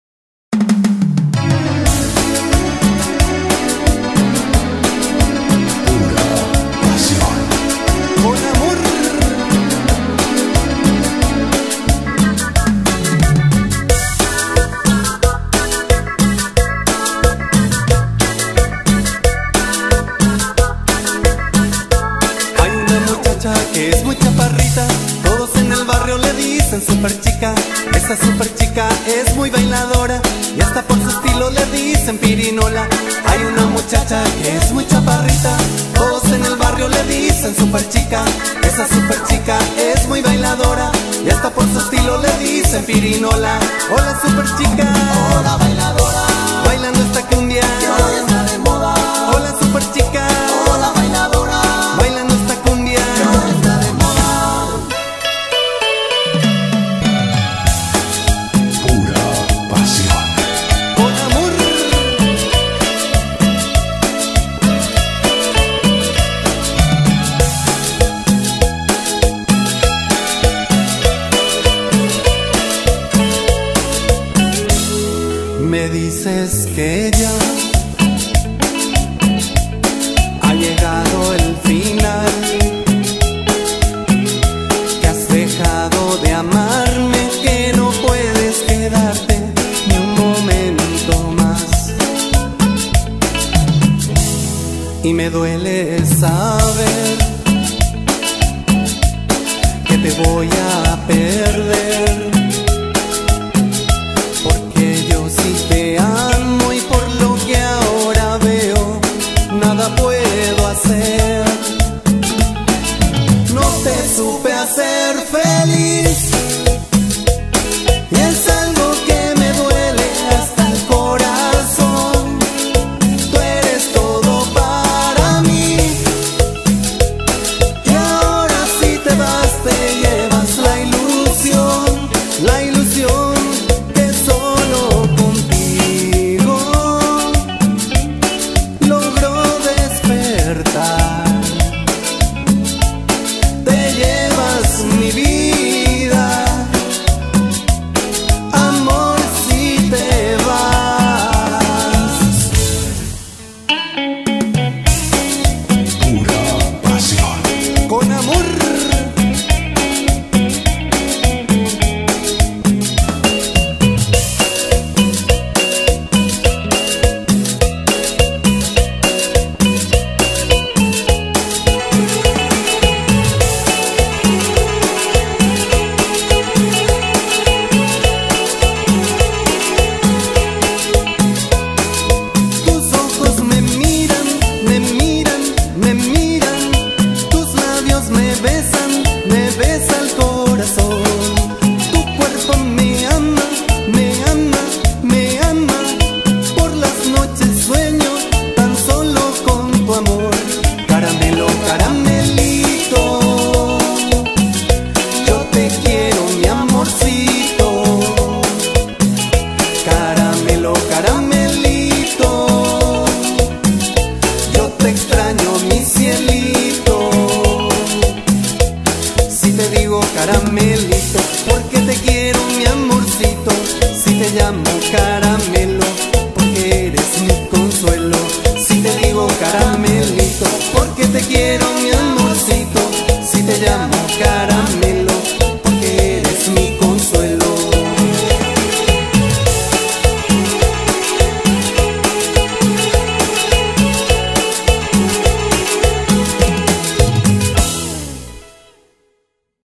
POPURRI CUMBIA- PURA PASION♫PABLO LISANDRO DJ ♫
POPURRI DE BUENA CUMBIA CON EL GRUPO PURA PASION,DISFRUTEN Y COMPARTEN EL VIDEO,APOYEMOS LOS NACIONALES